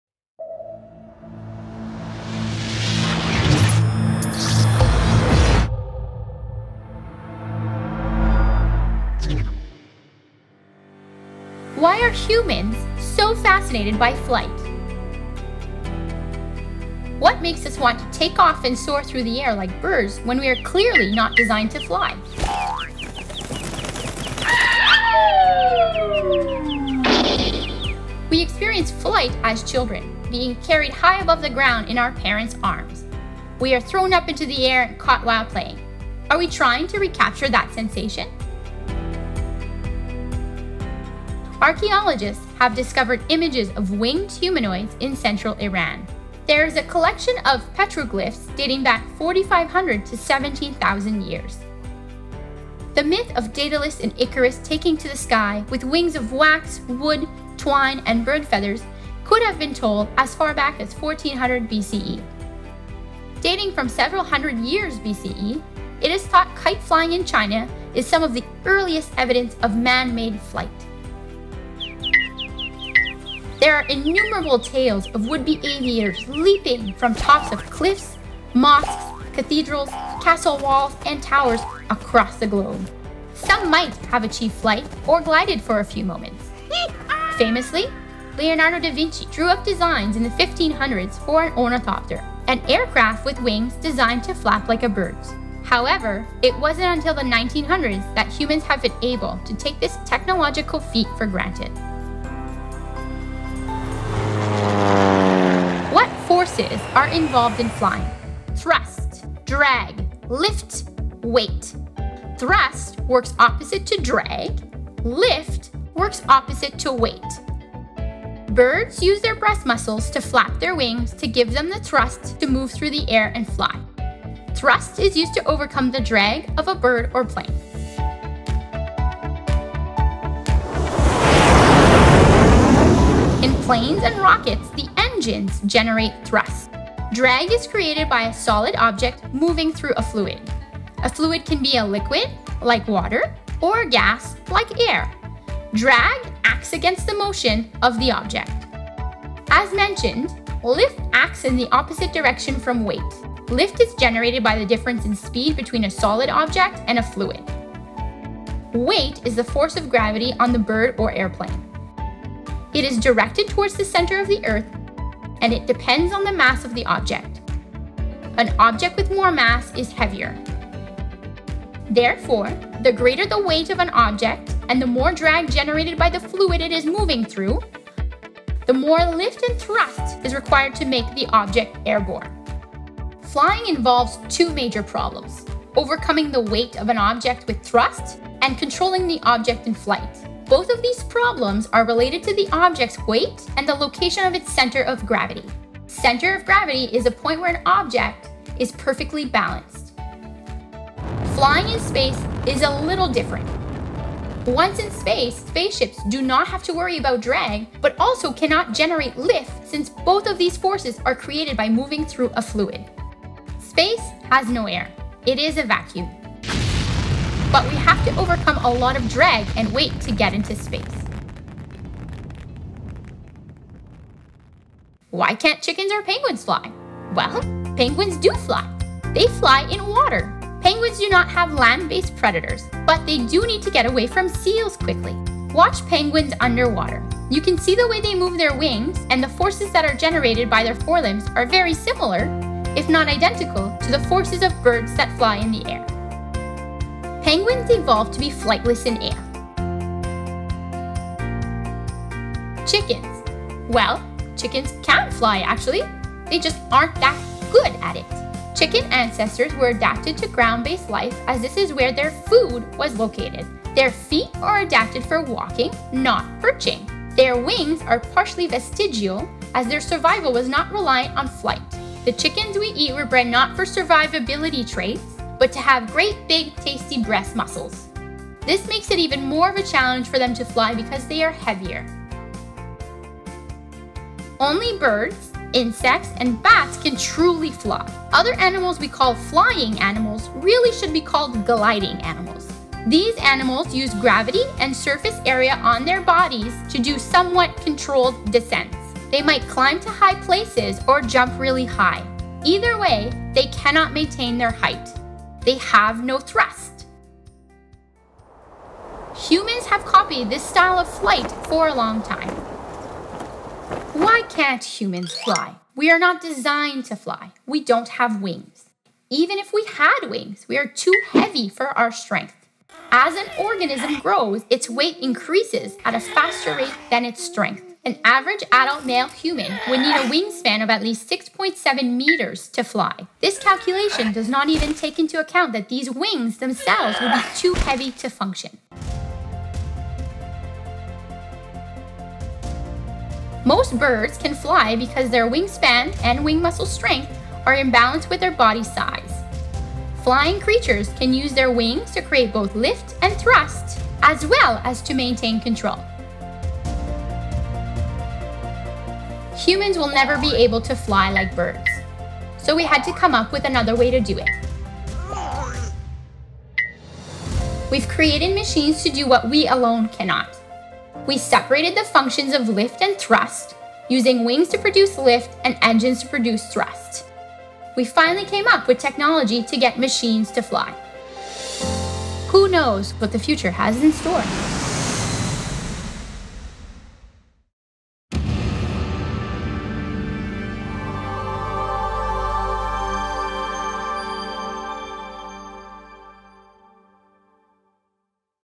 Flight - For the Classroom
Why can’t humans fly? Humans were not designed to fly but with technology we discovered a way to utilise the forces involved in flight to achieve flight.